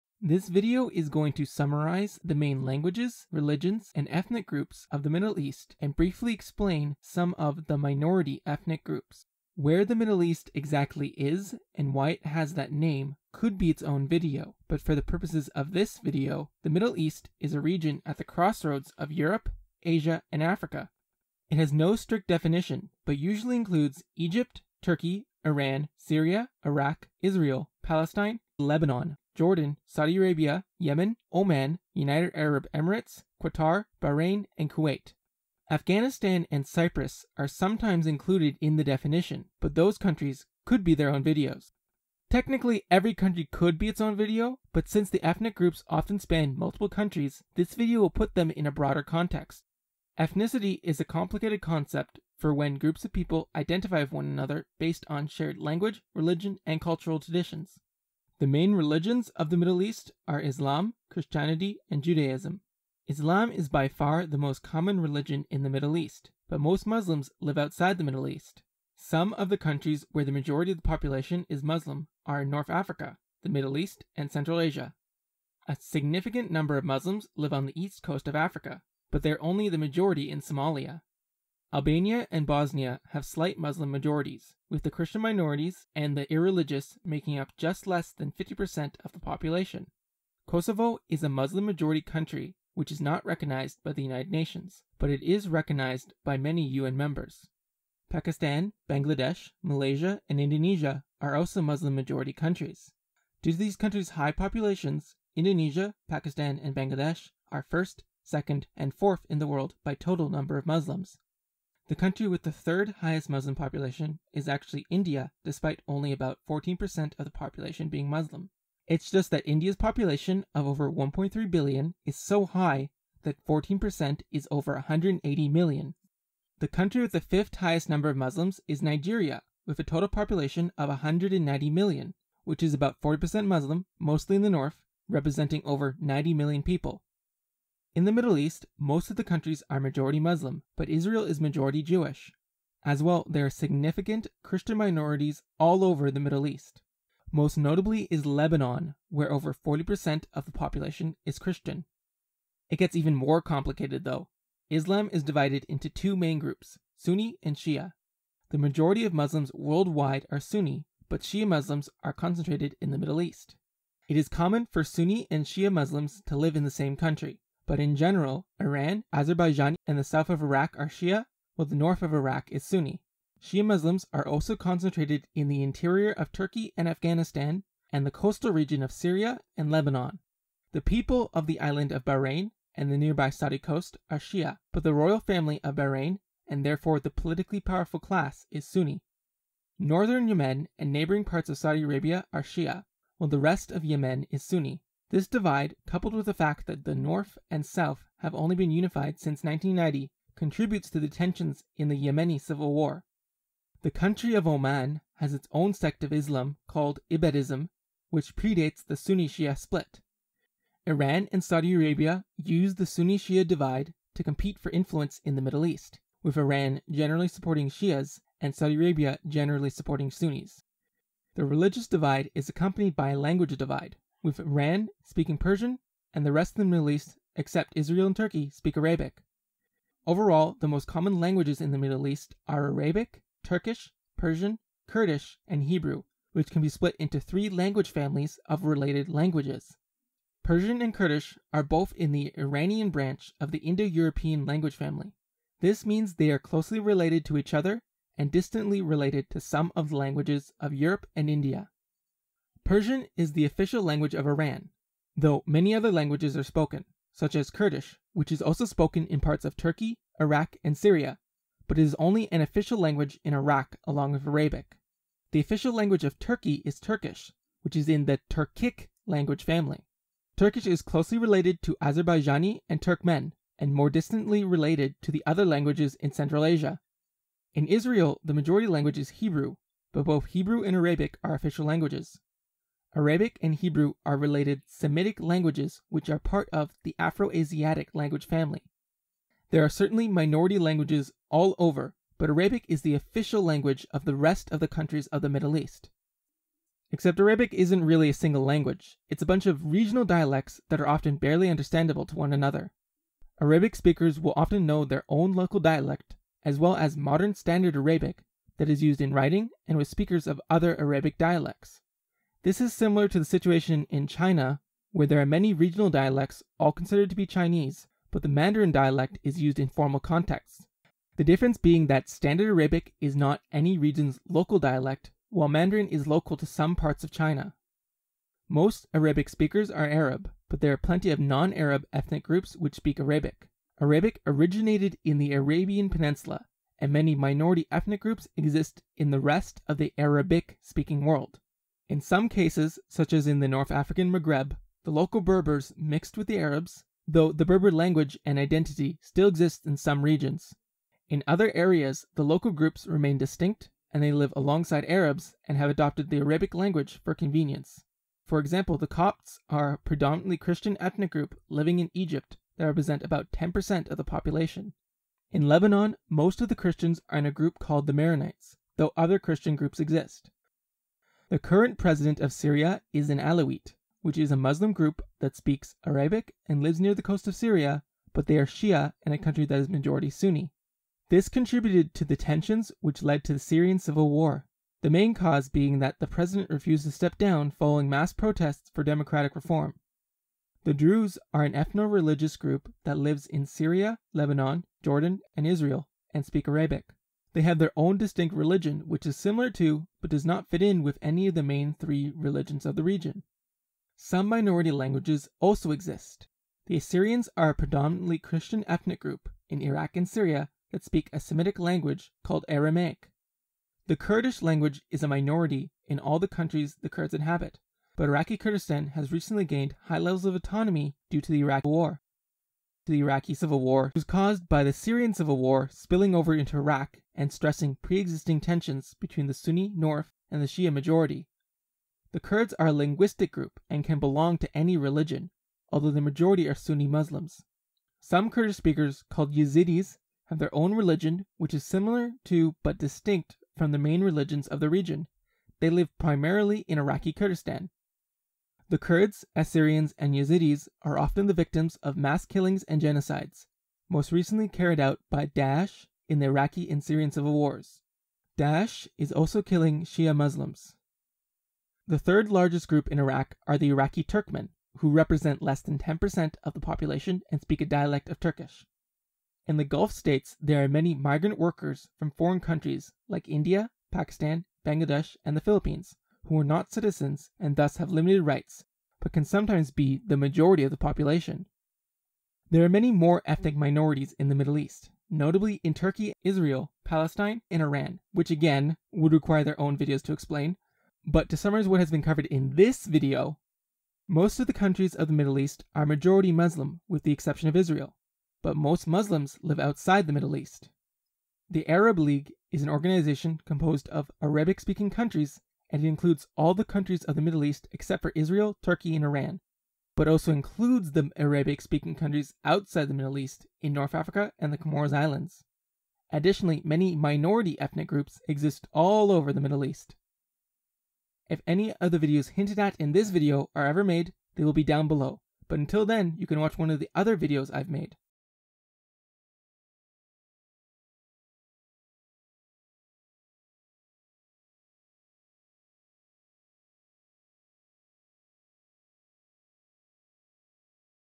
Middle East Explained - The Religions, Languages, and Ethnic Groups
The Middle East is at the center of everything, and is home to many ethnic groups which don't always get along.

The middle east is usually associated with muslims and arabs, but muslim arabs are only a part of the middle east. The middle east is also home to Turks, Persians, Kurds, Jews, Assyrians and many others.

Most of the countries of the middle east are muslim, but Israel is jewish, and Lebanon has a large christian minority. But islam is dividing into two main groups, the sunnis and the shias.

Shia muslims mostly live in Iran, southern Iraq, Afghanistan, Lebanon, Syria, Bahrain, and Northern Yemen, while Sunni muslims are the most common muslims worldwide.

Iran and Saudi Arabia take advantage of the sunni-shia devide to claim spheres of influence in the middle east, and many conflicts split down sunni-shias lines, such as the Yemeni Civil War and the Iraqi Civil War and the Syrian Civil War

Not all muslims are arab though. Turkey and Iran are definitely not Arab and speak Turkish and Persian respectively. Most arabic speakers are arab, though many non-arabs who live near arabs speak arabic for convenience such as the Coptic Christians of Egypt and Maronite Christians of Lebanon, and the Alawites of Syria.

The President of Syria is a Shia Alawite even though the majority Syria is Sunni Arab, this contributes to tensions in the ongoing Syrian Civil War.

One sunni group fighting in the Syrian Civil War called Daesh or ISIS or ISIL decided to invade neighbouring Iraq and started the Iraqi Civil War and fought against the Shia Arabs, and the Kurds in iraqi Kurdistan.